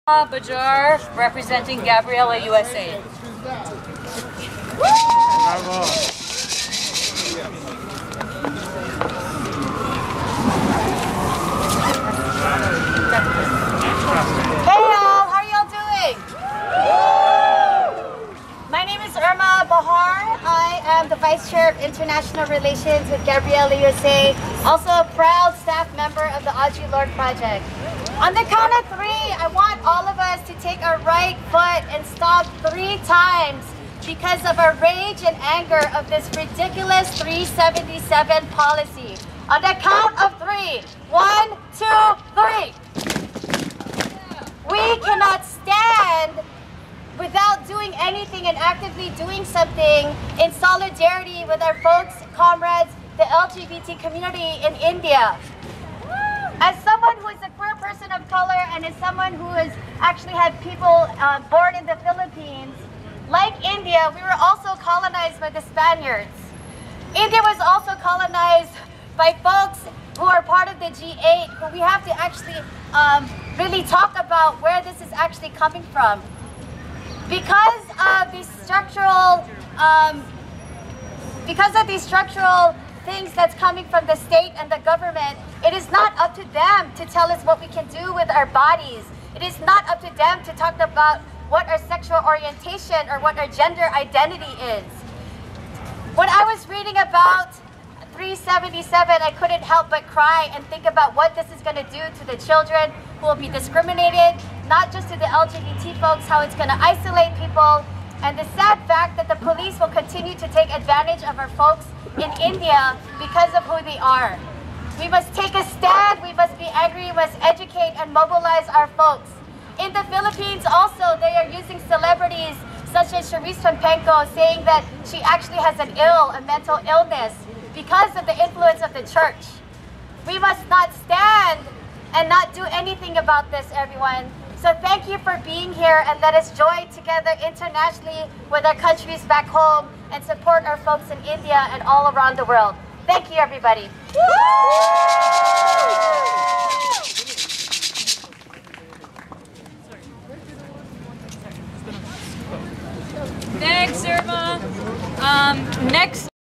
GABRIELA USA Global Day of Rage
All of Southern Asia looked to India as it's mentor into entering the modern global society with its values & standards regarding human rights.  The Philippines have a long history of being colonized, dominated & exploited.  A transman spoke up in NYC to give his perspective on India's decriminalization of its LGBT citizens.